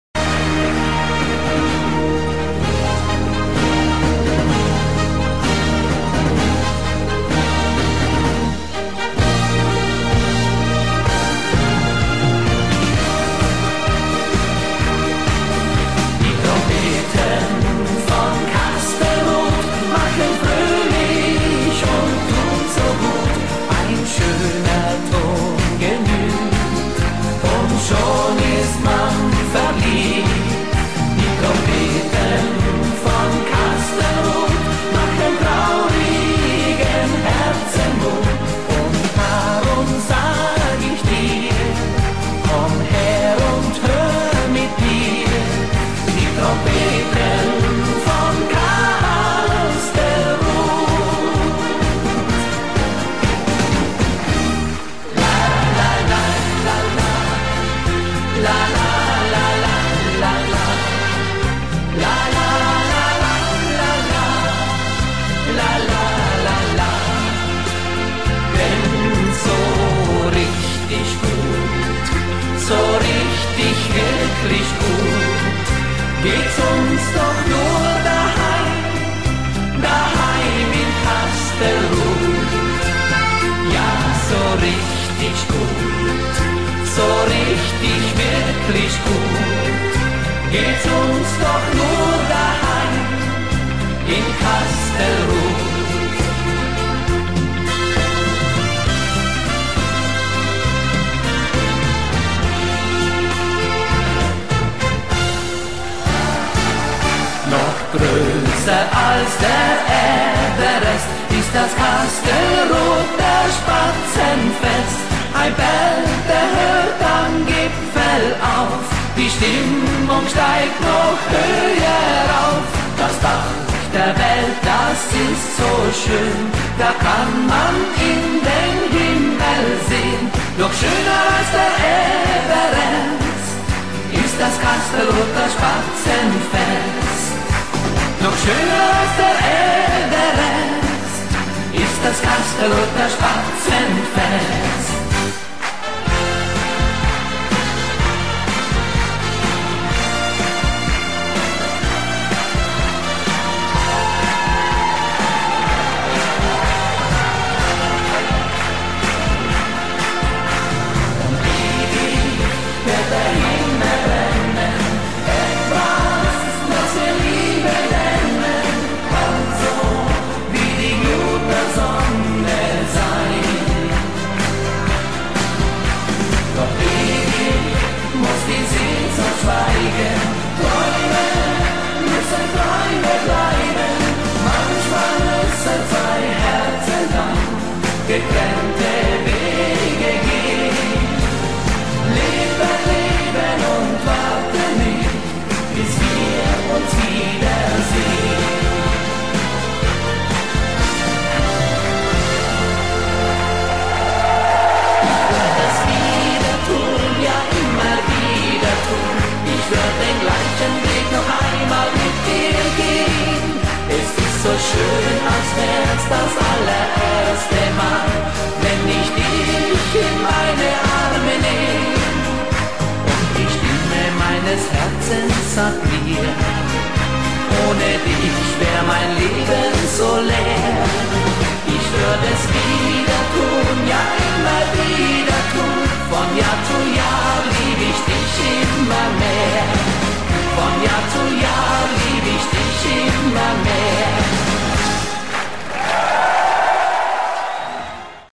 Tirol, Austria, 2010
We spend 3 days with my mother in Sonnwend Hotel, Fieberbrunn, Austria. That beautiful big mountins were beautiful.